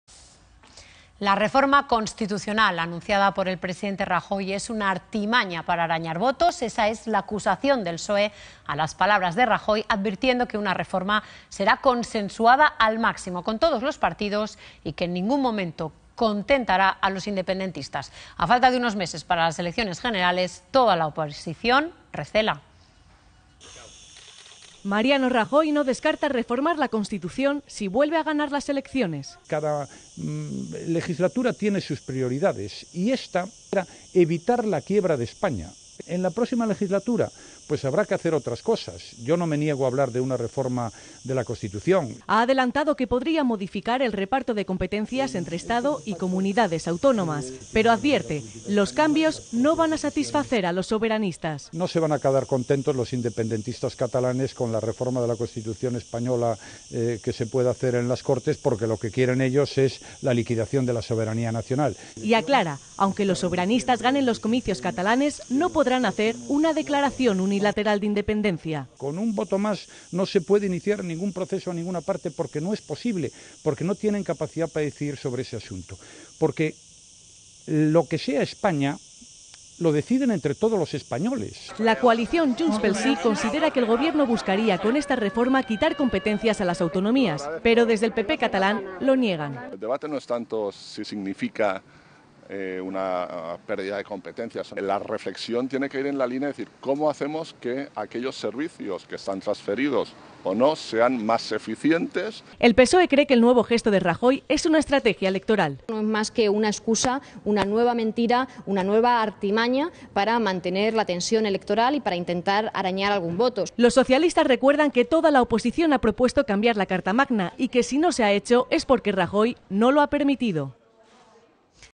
El PSOE califica de "artimaña para arañar votos" la reforma constitucional de Rajoy
La reforma constitucional anunciada por el presidente Rajoy es una "artimañana para arañar votos", es la acusación del PSOE a las palabras de Rajoy advirtiendo que una reforma será consensuada al máximo con todos los partidos y que en ningún momneto contentará a los independentistas. A falta de unos meses para las elecciones generales toda la oposición recela artimañana para arañar votos.